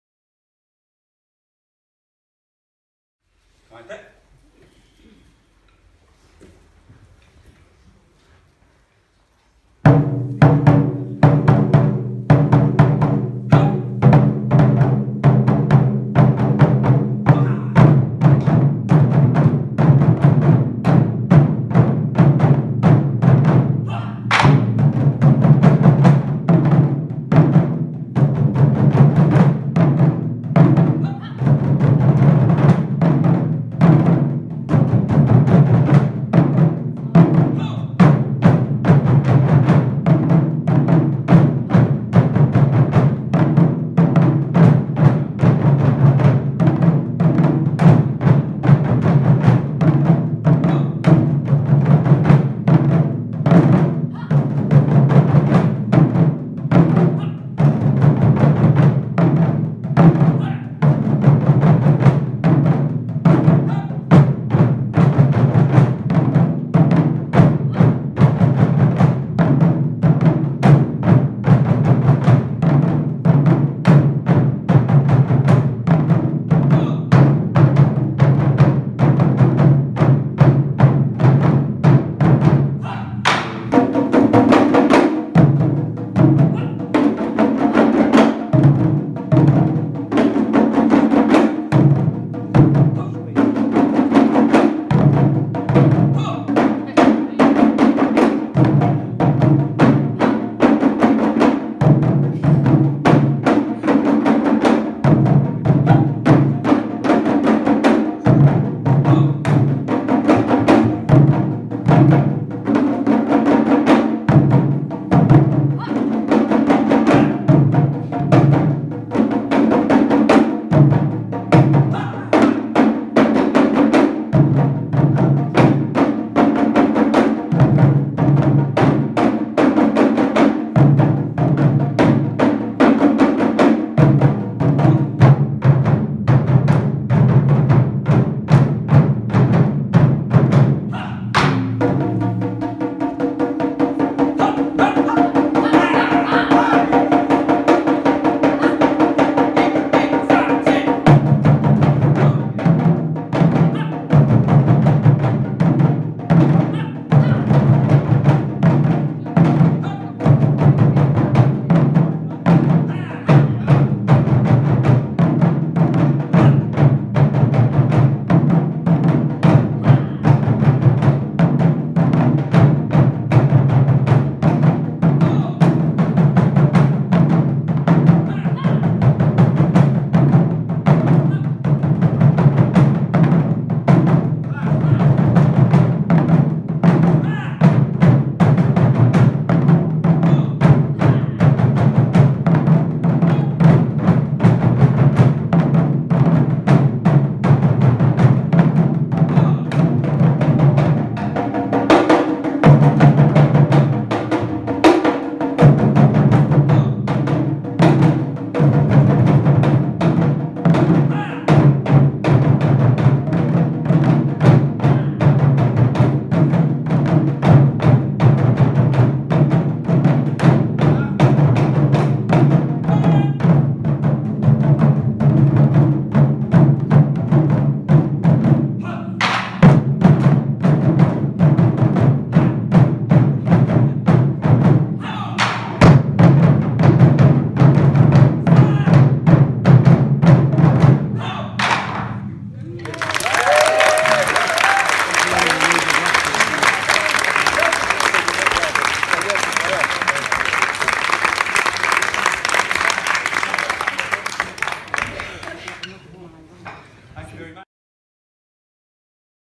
Midlands Taiko Day 30 March 2019 - Flow Performance - Taiko West
My first proper Taiko performance in front of a large audience with my weekly group - 'Wednesday Church Stretton Adult Taiko Drumming Group.' Thoroughly enjoyed it!